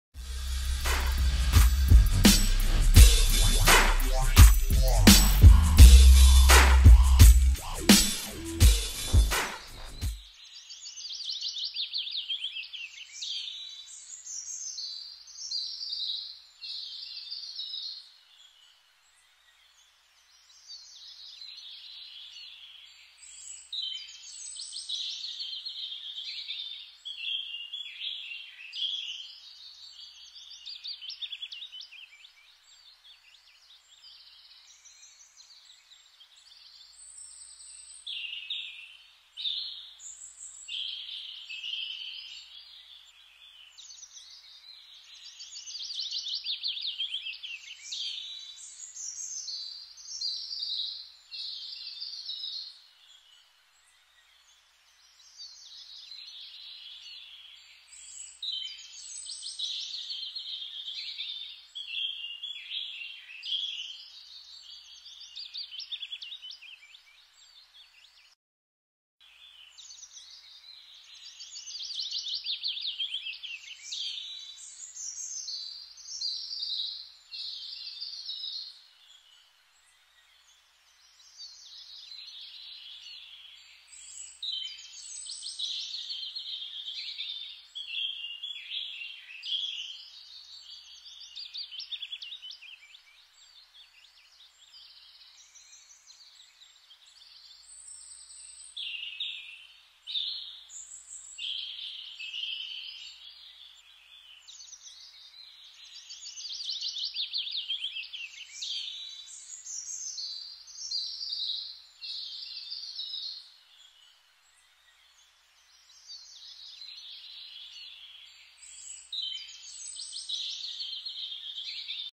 S.H.Figuarts Nebula Bandai SHF Avengers Infinity War Action Figure Product Shot Review
S.H.Figuarts Nebula Bandai SHF Avengers Infinity War Action Figure Product Shot Review - recently announced and up for preorder at many retailers. Due to release September 2019